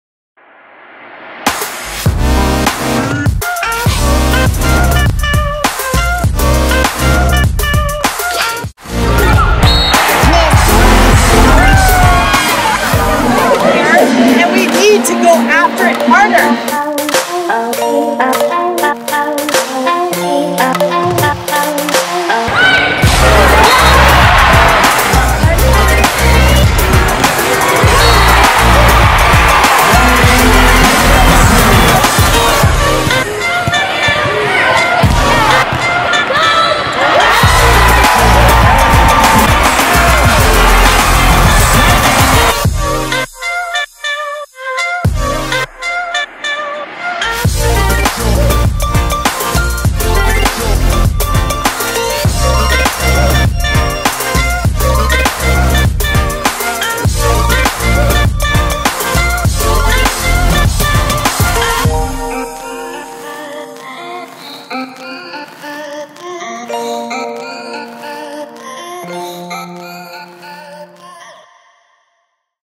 University of San Diego Volleyball 2019
The University of San Diego was the West Coast Conference Champions for the 11th time and made its 23rd NCAA tournament appearance